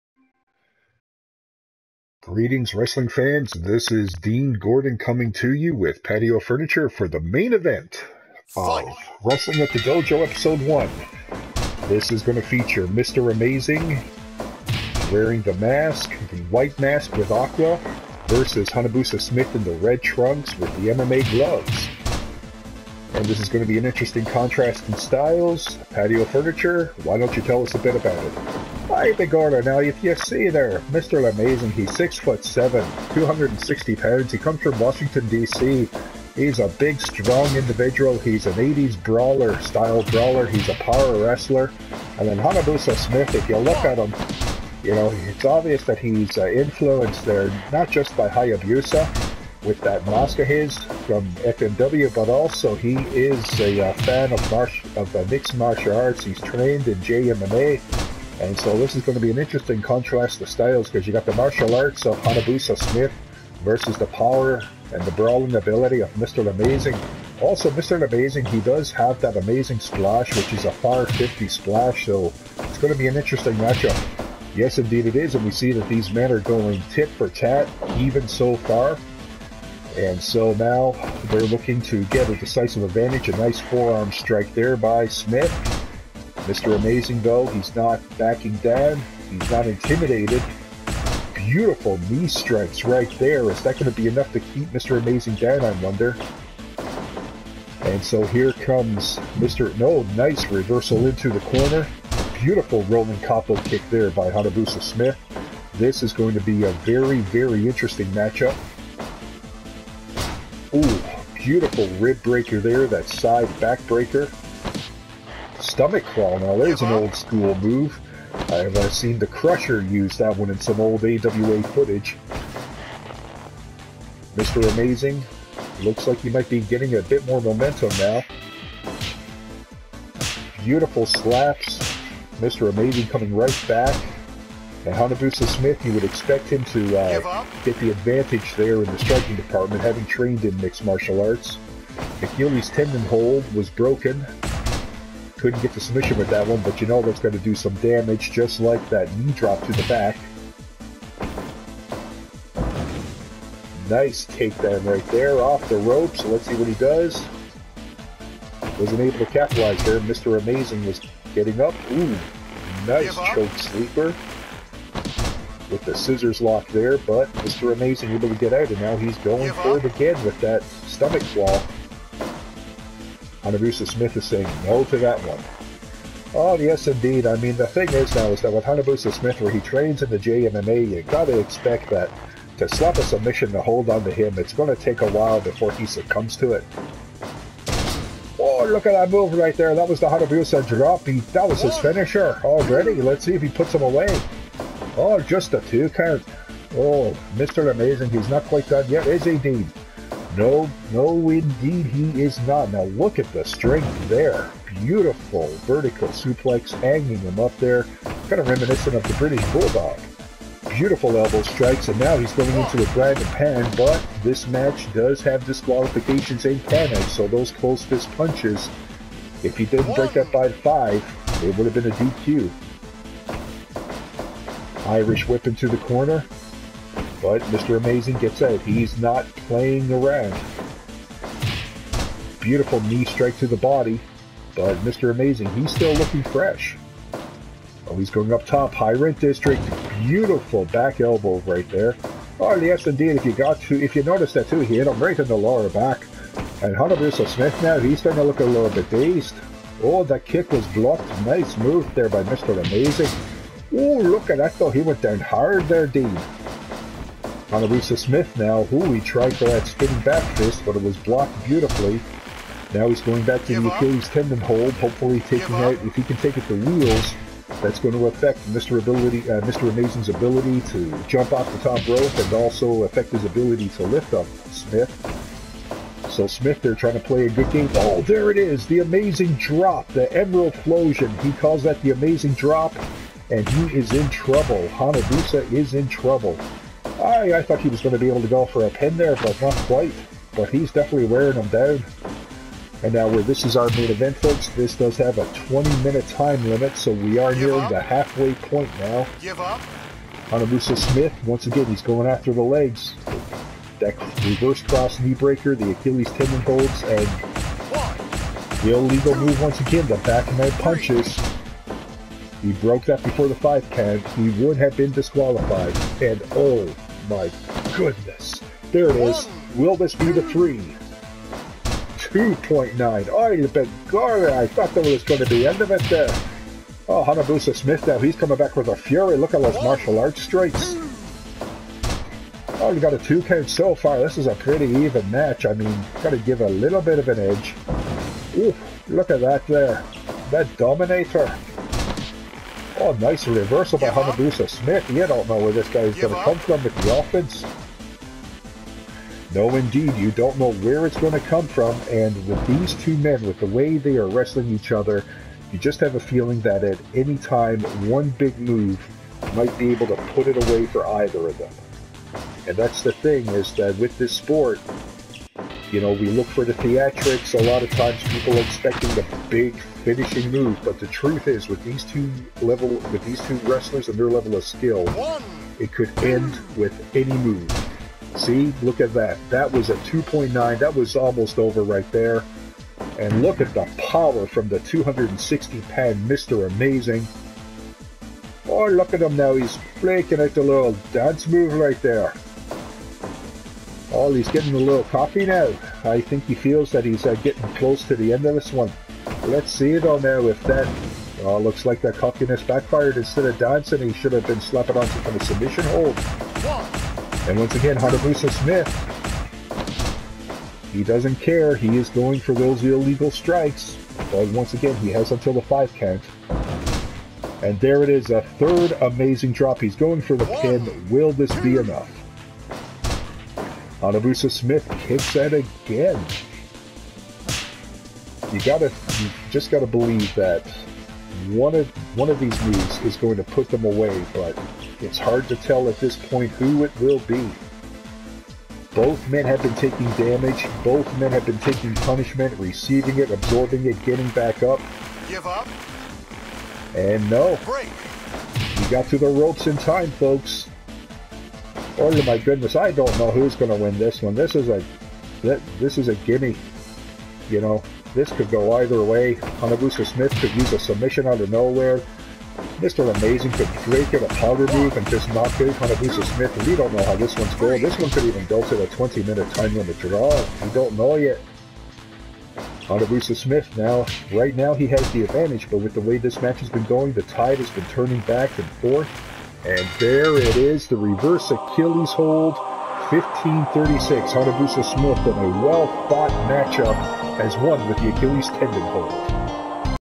PWF Wrestling at the Dojo Ep 1 Match 5 Mr. Amazing v Hanabusa Smith
Main Event! 1 Fall, 20 minutes, DQ and Countout Enforced.